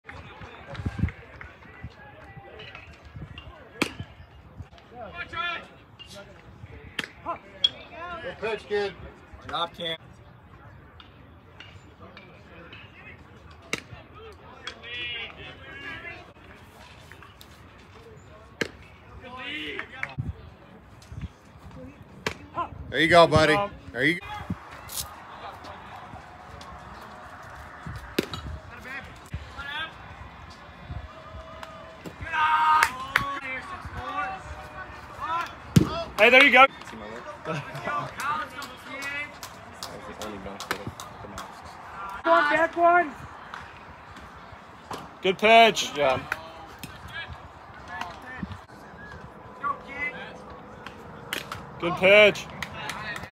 Kam Yearsley Pitch PBR IL Summer Champs 6/23-6/26/22
Kam Yearsley @FoxesBaseball class of 2024 w/ 6 IP, 4 R, 4 BB, 4 K and 1 win this weeekend. FB 77-79, has T82 mph, CV 65-67. Competitor on the mound! ESPTrained @TaddGibby24 @KameronYearsley @PBRIllinois @PG_Illinois @PBR_Uncommitted @FlatgroundApp @TopPreps @AndySrokaPBR